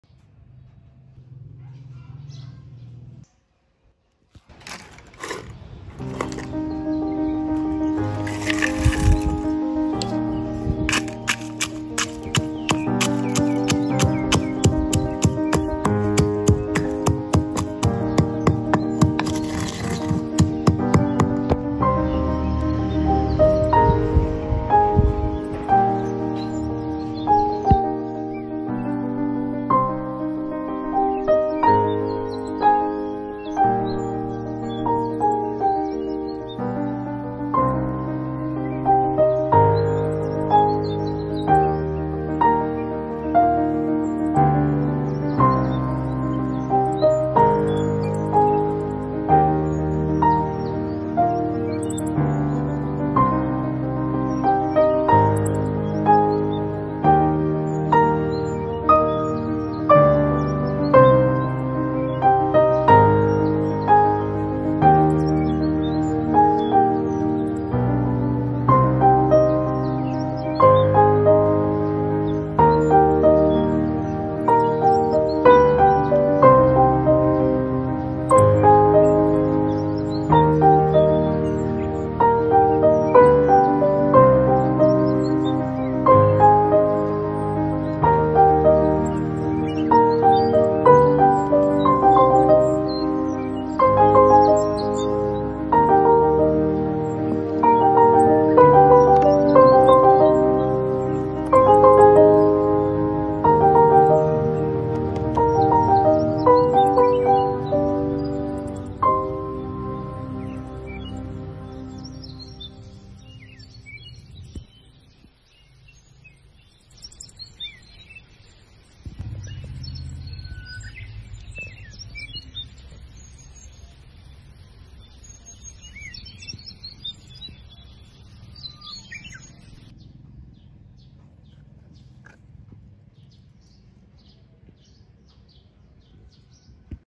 Tip for the Fruit Grow Faster and Bigger by adding this from the Kichen | Garden Famil
Tip for the Fruit Grow Faster and Bigger by adding this from the Kichen
 Hope you like this tips for your garden, growing faster to harvest.
My smaill garden share.